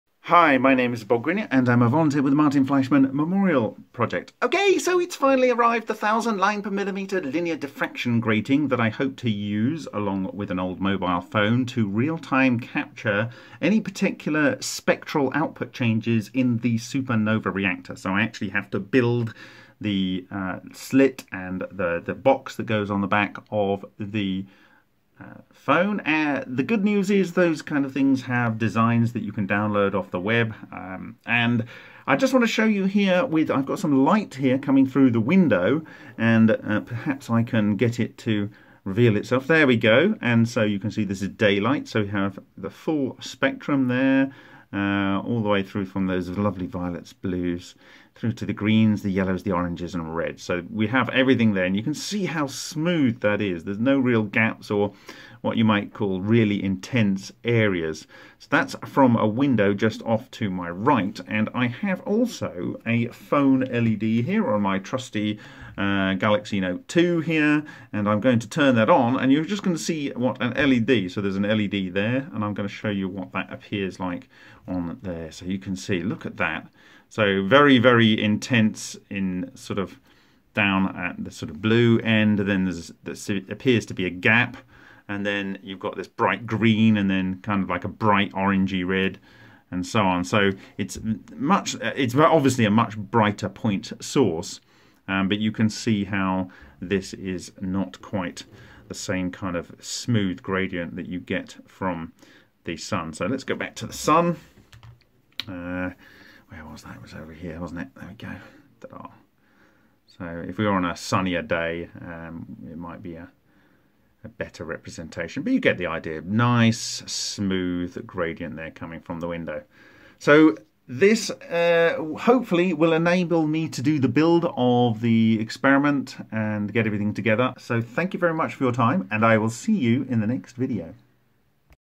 Super NOVA - Diffraction grating
The 1000 line diffraction grating, intended to be potentially used for real-time spectral analysis of the Super NOVA plasma, is subject to a basic test.